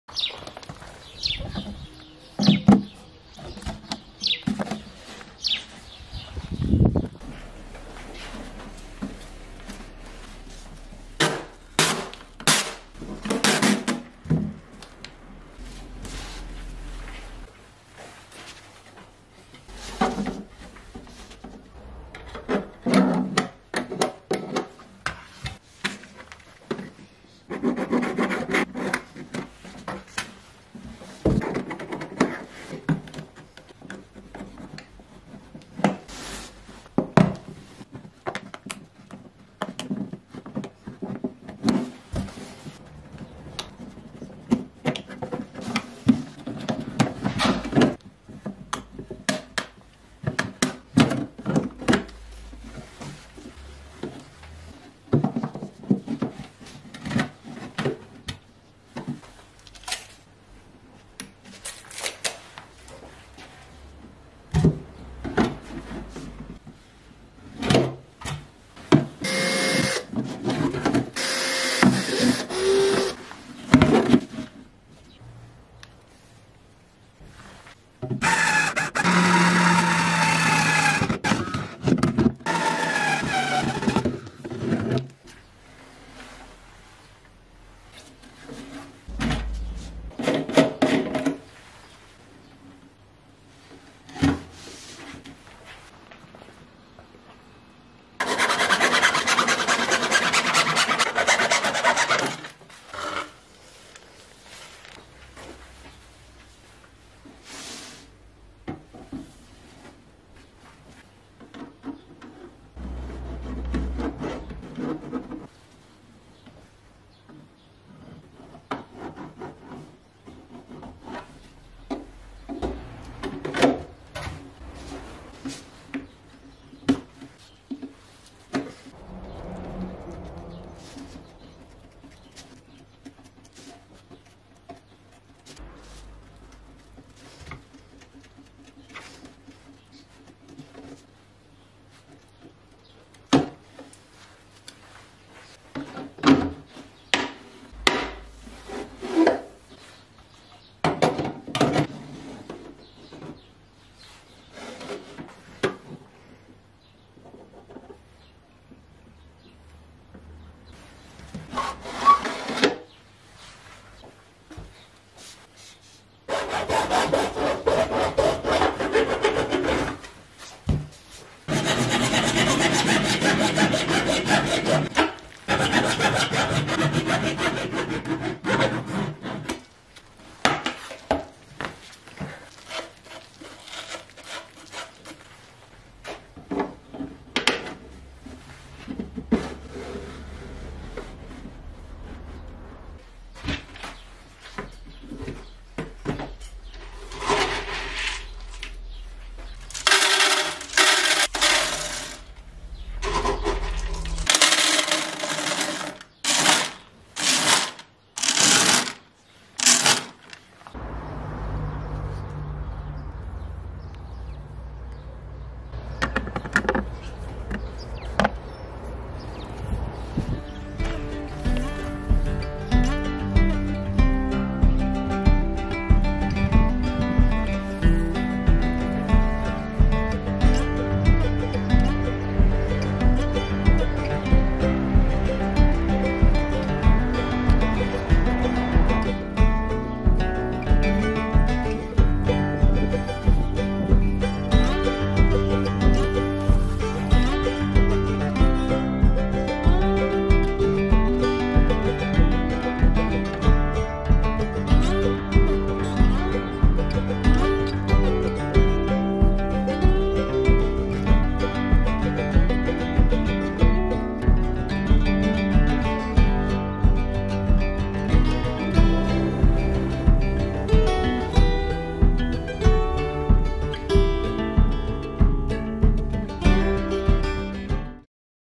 Como hacer un Comedero para Gatos Casero
En el vídeo de hoy fabricamos un comedero para gatos o perros utilizando materiales reciclados